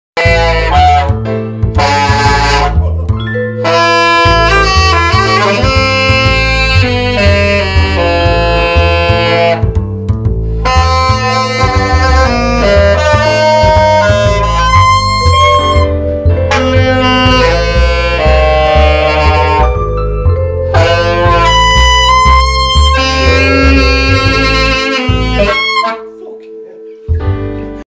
Jazz Legend - Chris "The Sax Machine" Roberts
Epic smooth Jazz improv by the master of the sax